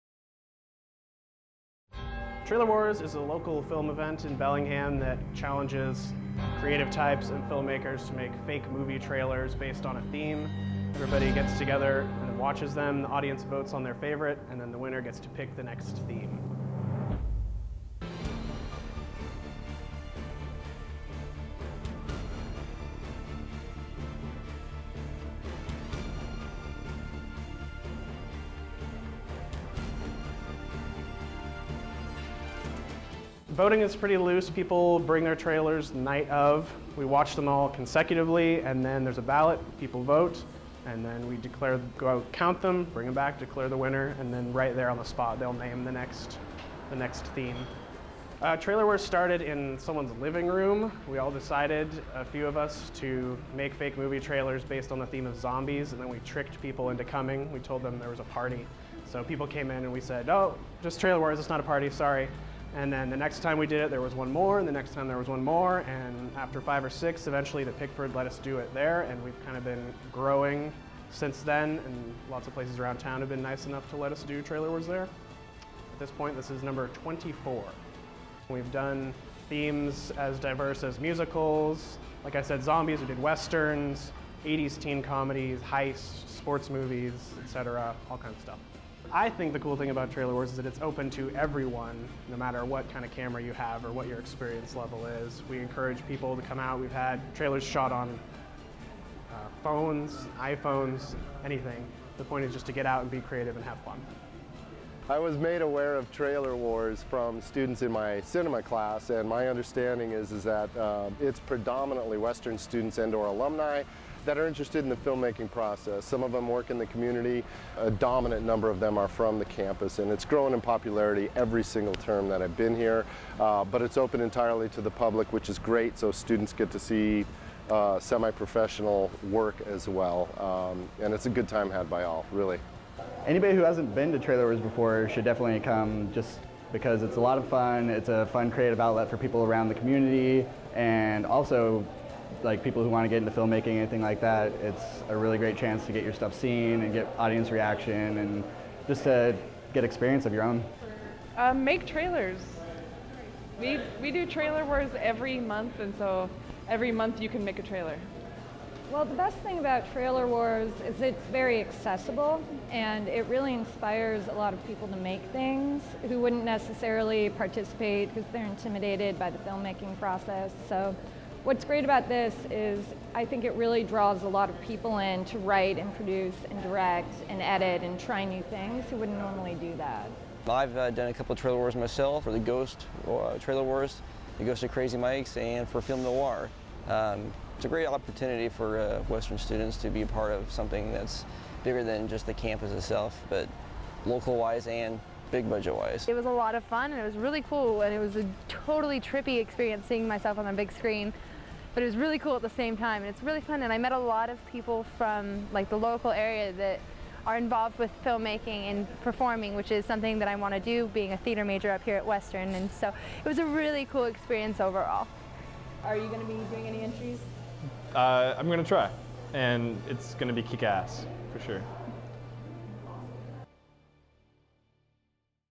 CNM: What is Trailer Wars?
This video talks about Bellinghams's local film festival, Trailer Wars - a competition where people create trailers to non-existent movies based on a rotating theme.